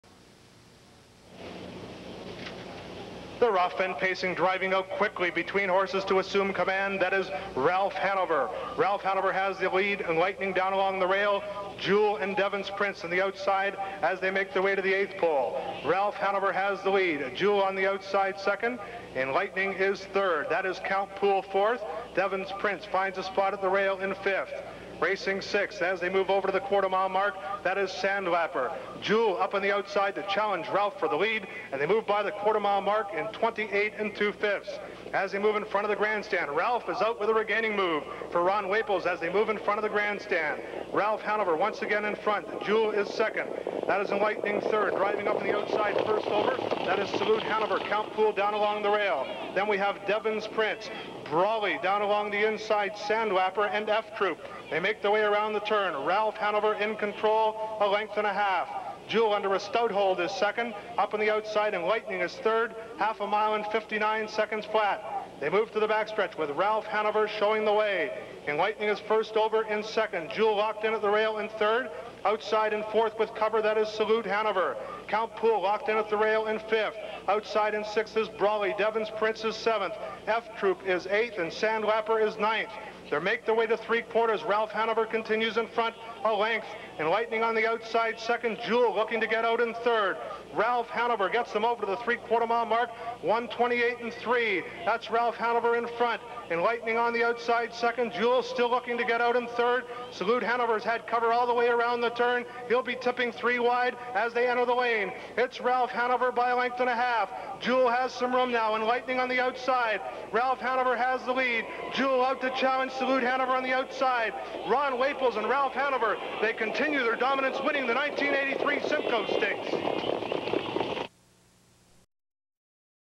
1983 Mohawk Raceway - Ralph Hanover & Ron Waples - Simcoe Stakes
$109,160

Joule, Devons Prince, Enlightening, Count Pool, Sandlapper, F Troop, Brolly, Salute Hanover